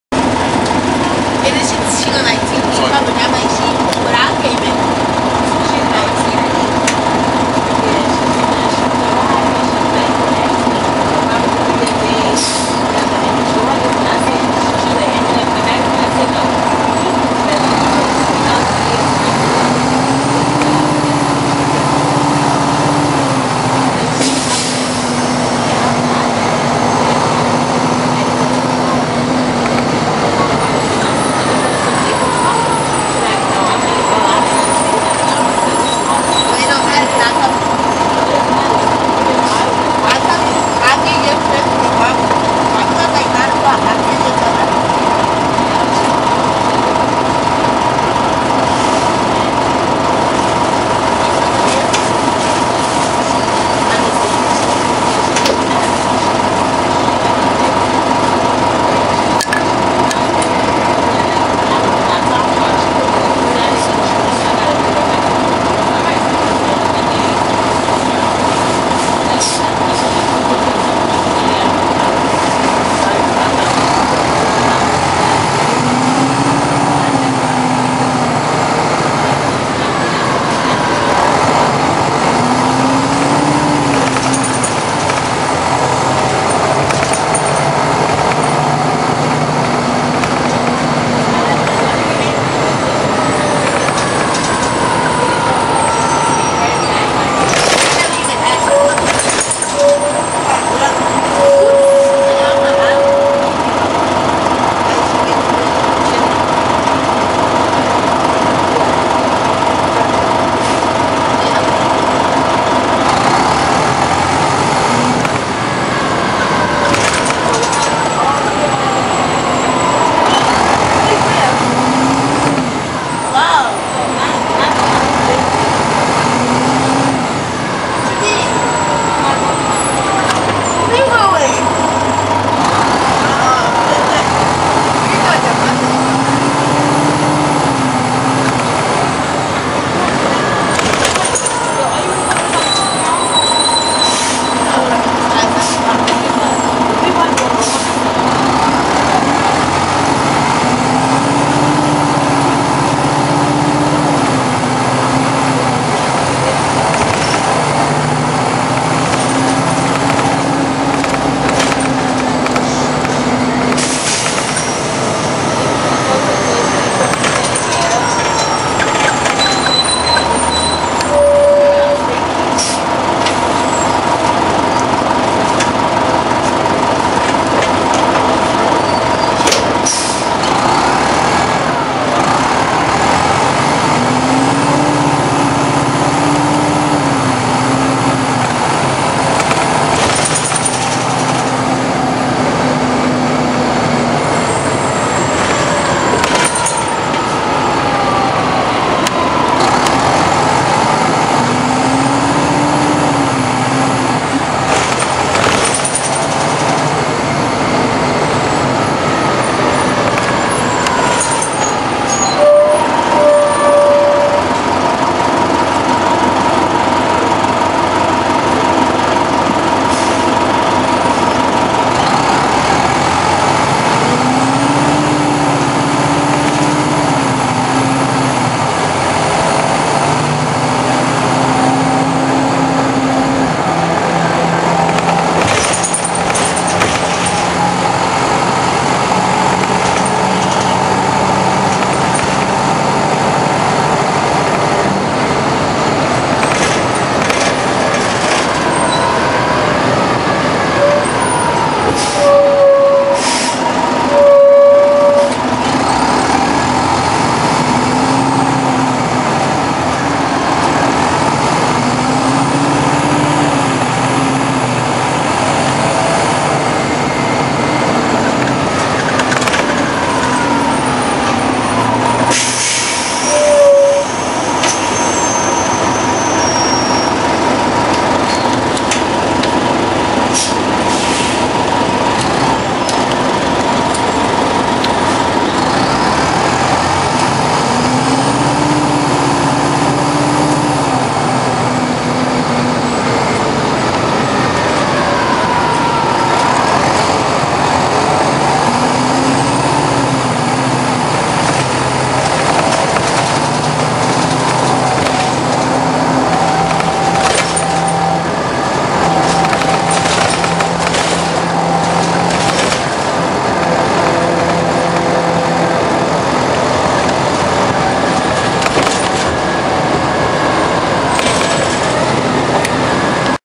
1999 RTS NovaBus #5123 B6 recording!
Made my 14th RTS NovaBus recording. This is a good bus and sounds really good, the B/O was trying to make this bus run hard...hope you like!

Year: 1999
Maker: NovaBus Corporation
Model: T80-206 RTS
Engine: Detroit Diesel Series 50 DDEC
Transmission: ZF ECOMAT-2 HP-592

The following people that was with me during the making of this video was rts9100, TheTransitMan123 and Allen1628famm